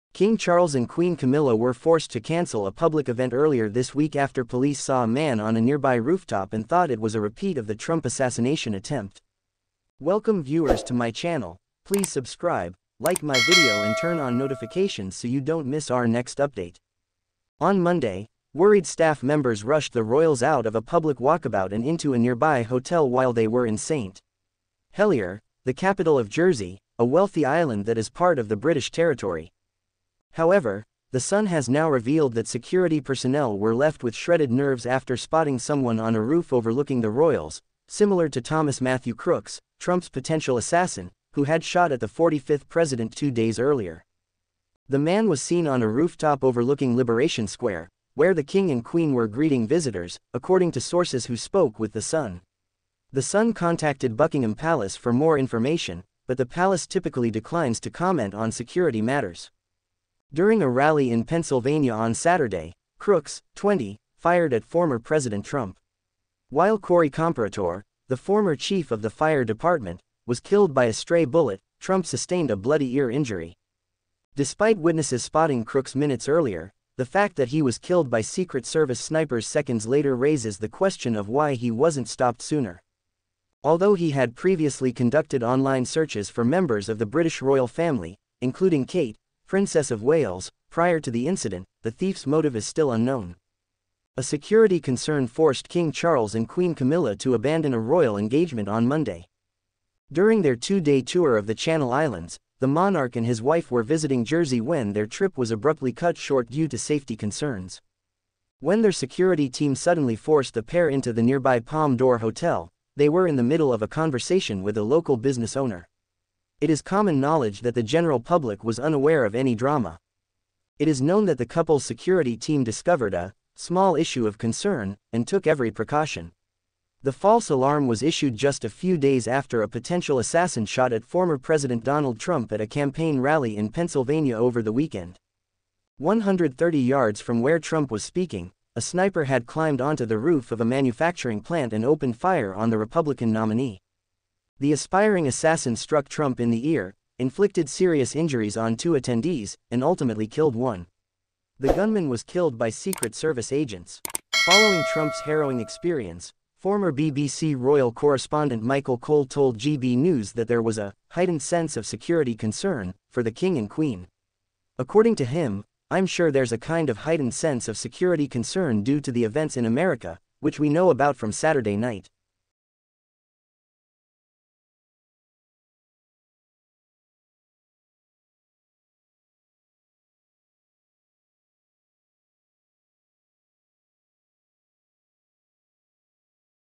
King Charles and Queen Camilla rushed to safety after a sniper was spotted in rooftop near an event
King Charles and Queen Camilla were forced to cancel a public event earlier this week after police saw a man on a nearby rooftop and thought it was a repeat of the Trump assassination attempt."